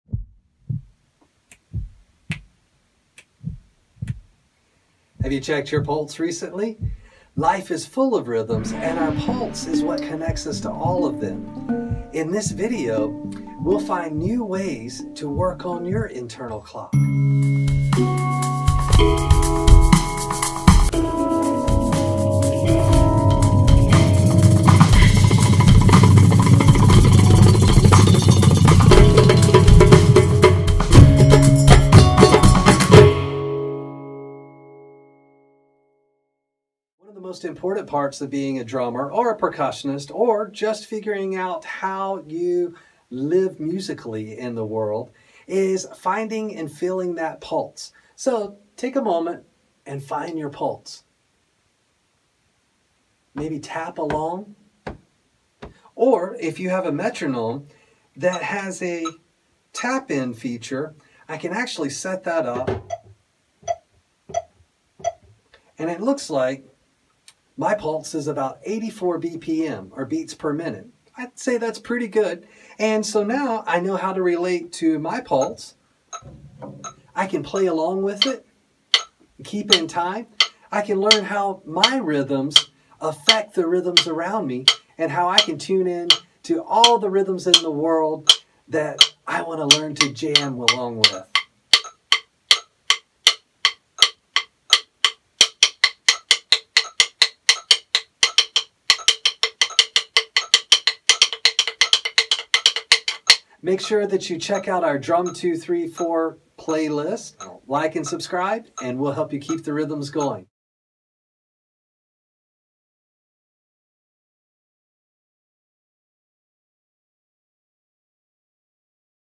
Check Your Pulse - DRUM234 with Jeff Holland Sonic Artist
Have you checked your pulse recently? I hope your rhythms are doing well. We all have to find our place in time, and your pulse connects you to all the rhythms of life around you. Tune in and Tune up your biological clock by setting a metronome to your heart rate. This will connect how you feel with how you play. Try some simple patterns to compliment your heart beat, and keep your rhythms going! Be safe. Be well. Be creative. In time.